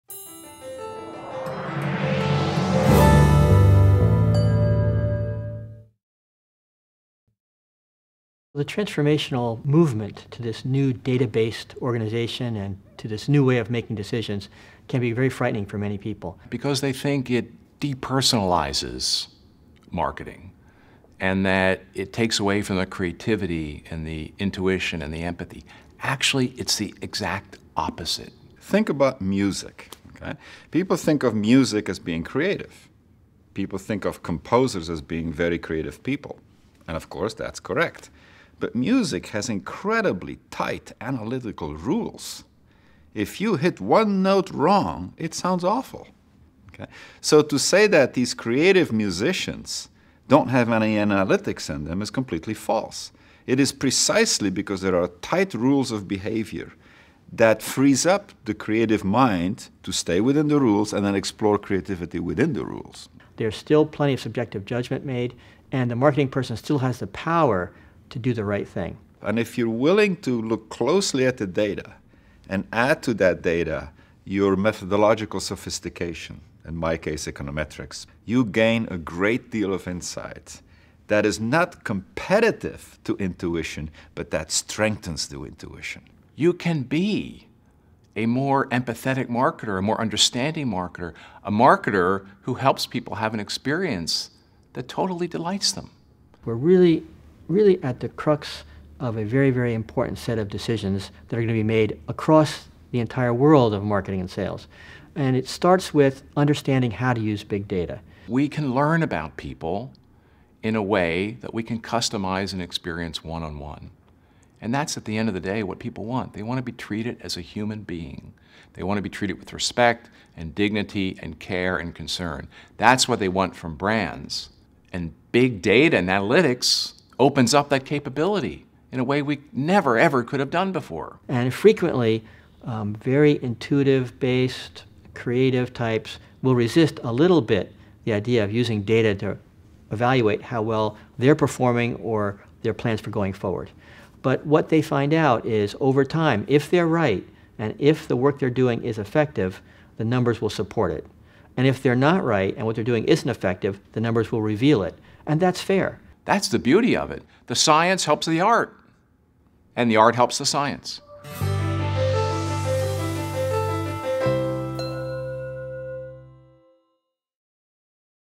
Art vs Science in Marketing Analytics
Learn how art and scienceare coming together through marketing analytics -- and allowing marketers to perform at a higher level.